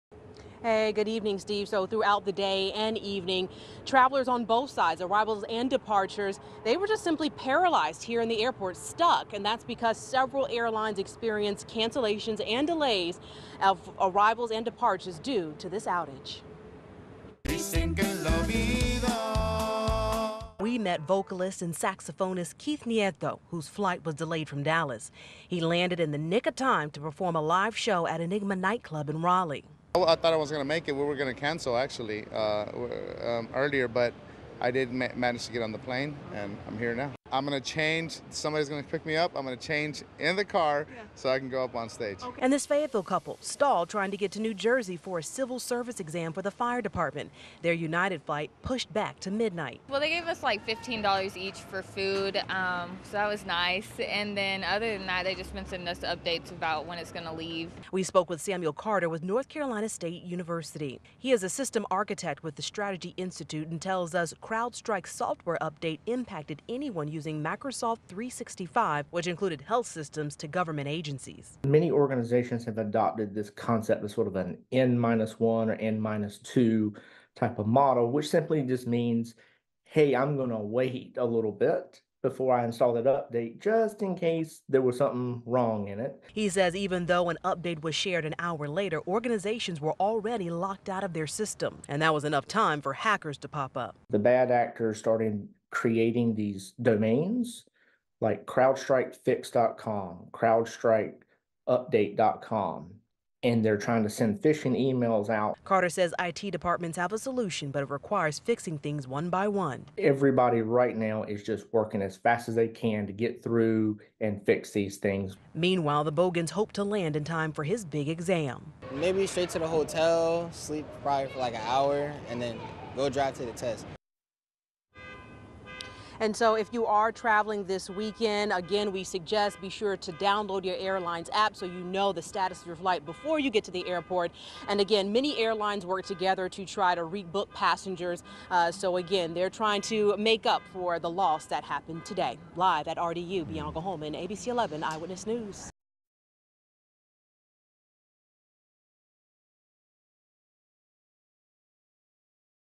Fallouts continues for travelers after largest global IT outage
Travelers were left frustrated and stranded at airports after Crowdstrike said it issued a routine Microsoft Windows software update that had a bug in it.